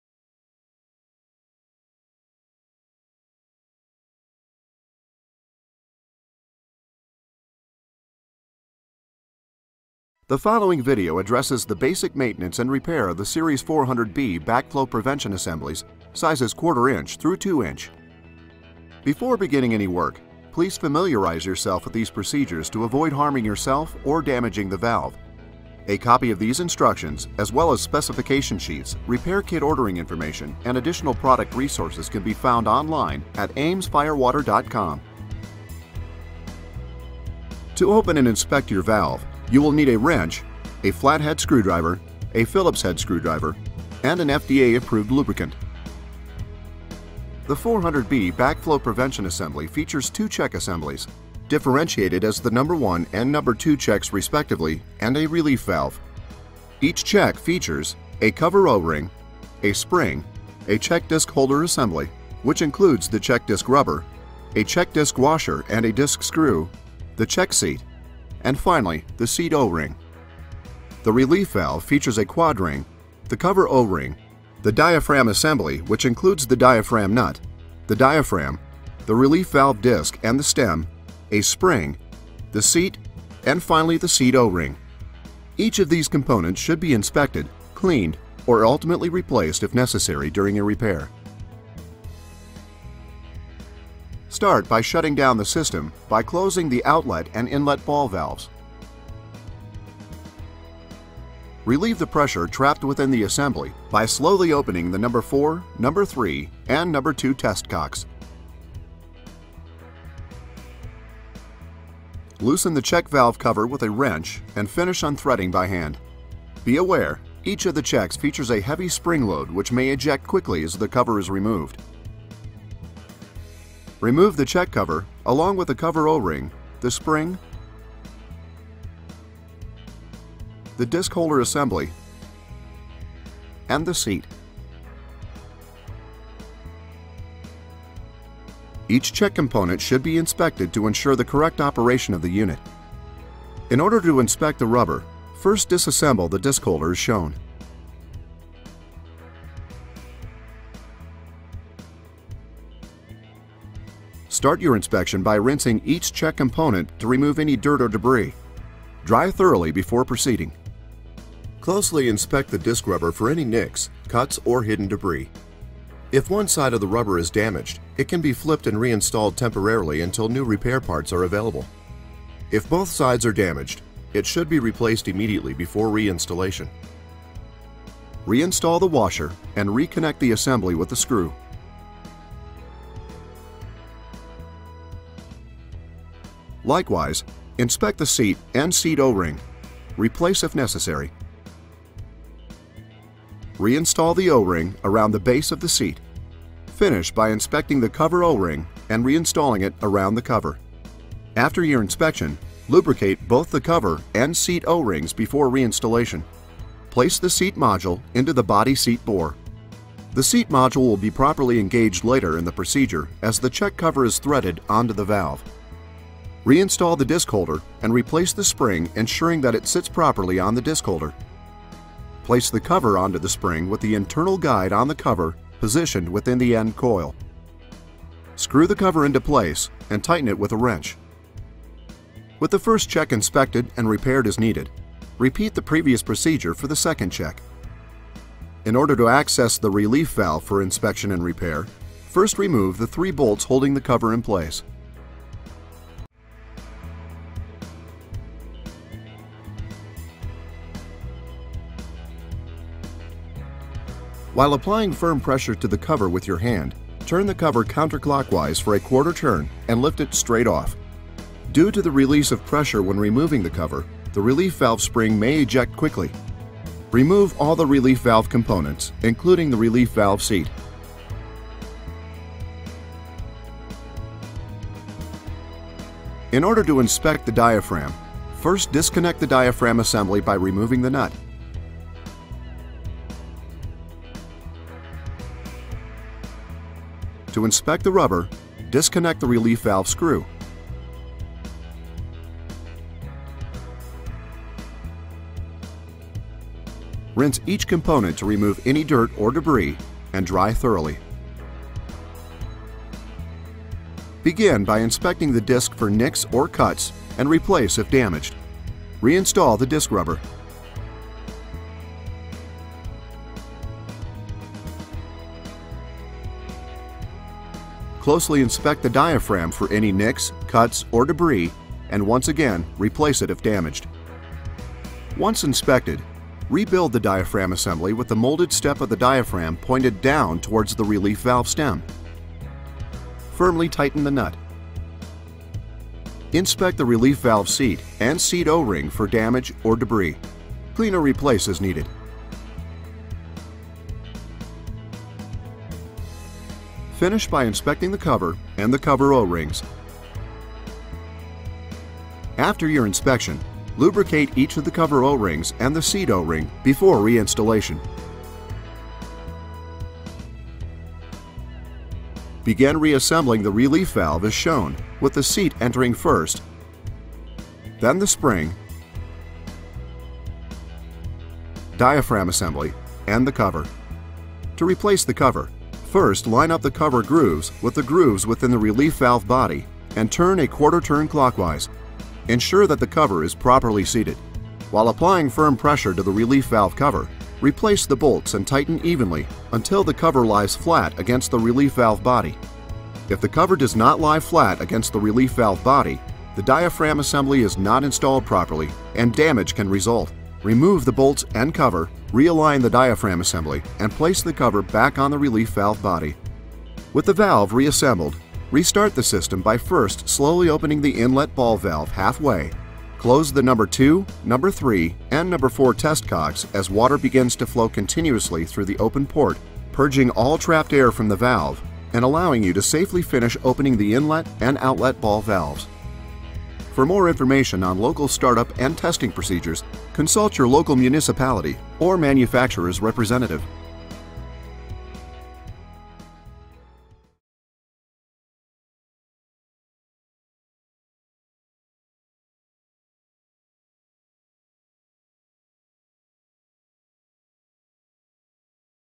Series 400B ½"- 2" Backflow Preventer Repair | Watts Repairs
Join the experts at Watts as they walk you through the inspection and repair the Series 400B backflow preventers, sizes ½" - 2". This video covers basic assembly maintenance as well as cam-check inspection and repair.